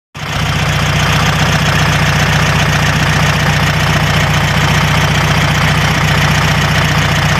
1974 Superbeetle leaking fuel pump.
I started Greenie up today and saw that it was leaking lots of fuel. This is an engine fire waiting to happen. Greenie already survived one engine fire in the 80s which is why it does not have the original engine. So either I have it towed to a shop that can work on it or I try to DIY replacing the fuel pump.

Anyone in Columbus Ohio area want to help? I will provide pizza, beer, and monetary compensation.